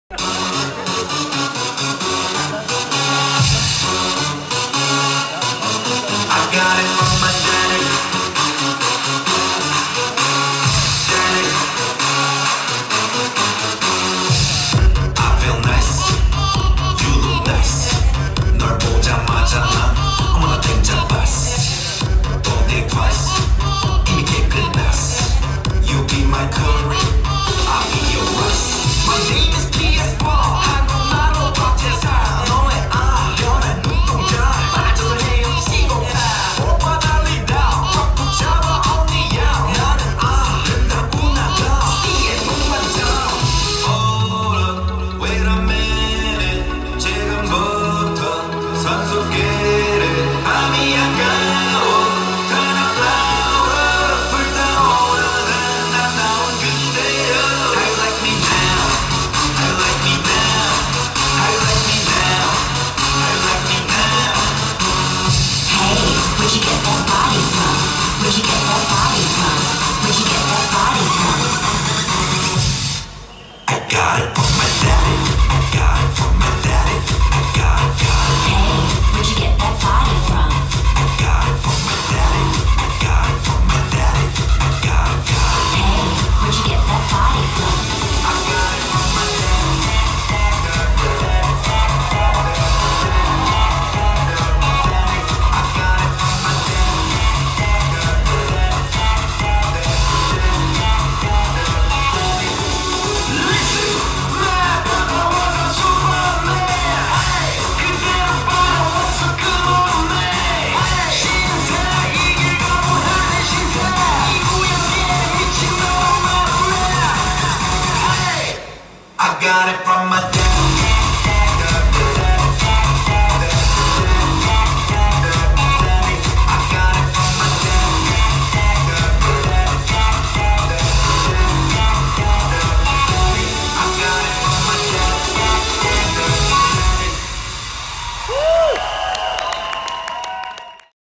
Great Eastern Annual Award 2019 - Kpop Dance Performance
"Kpop Dance Performance"
Danced by Ultron Entertainment's Dancers

Great Eastern Annual Award 2019
Genting International Convention Centre